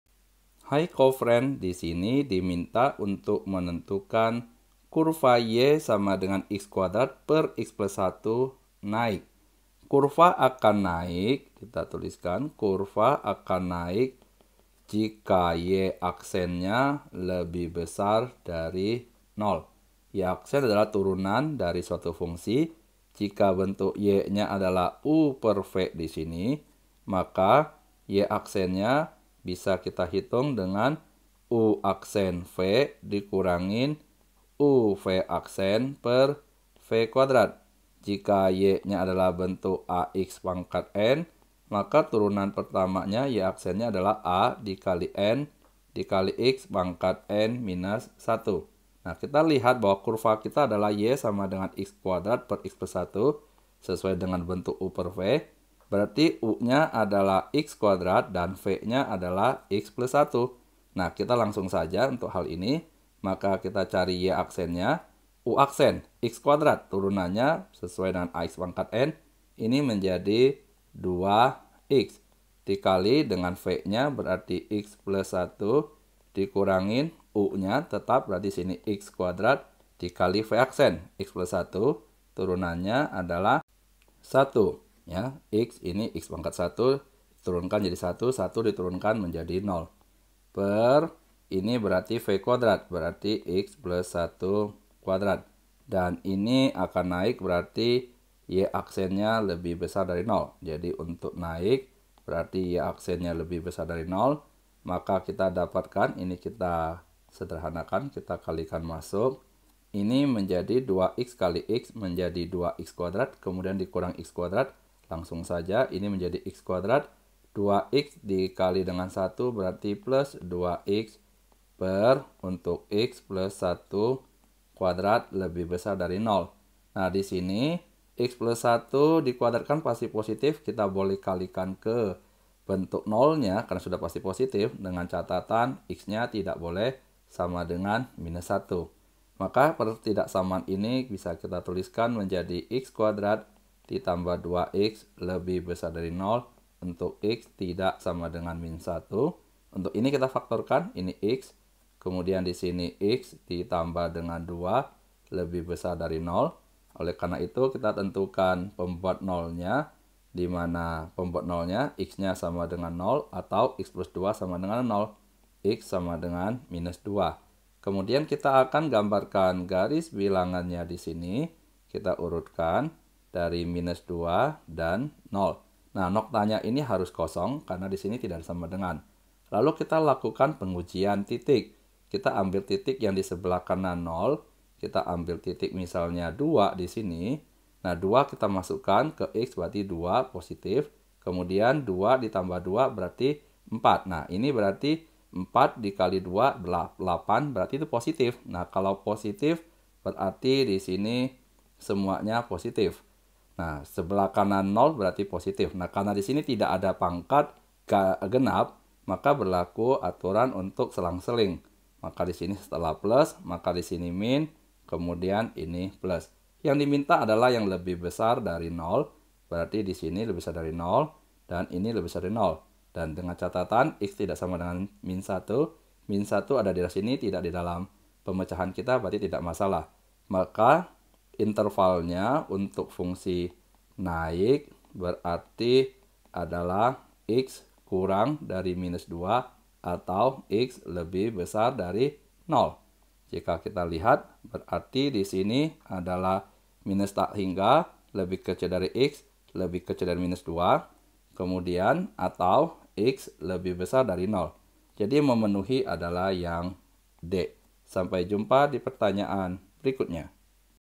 Kurva y=x^2/(x+1) naik pada ....(A) 2&lt;x&lt;-1 atau x&gt;0 (B) x&lt;-2 atau -1&lt;x&lt;...
Topik atau Materi: Fungsi Naik - Turunan - KALKULUS - Matematika Kelas 11 SMA -

Sumber Soal: Matematika Zenius Semester 2 , N/A

00:00 : Bedah Soal
01:00 : Konsep, rumus dan pengertian Fungsi Naik
01:18 : Langkah penyelesaian soal
03:20 : Jawaban akhir
03:50 : Penutup